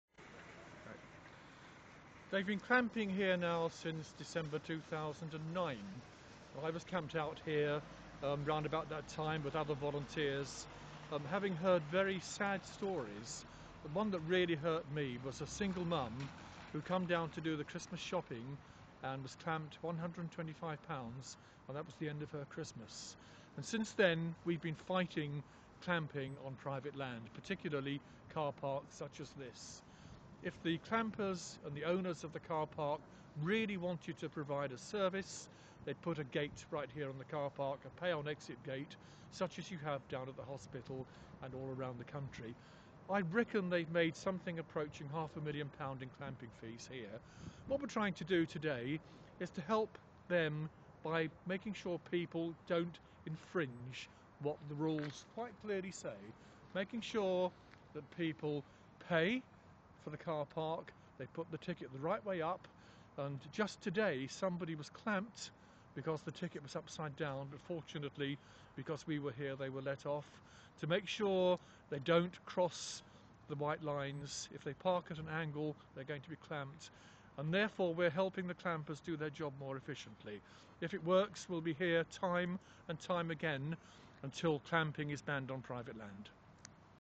Cedar Lane Carpark Action Group - Cllr Denis Fuller
County Councillor Denis Fuller explains the activities of the Cedar Lane Action Group working outside Cedar Lane Carpark today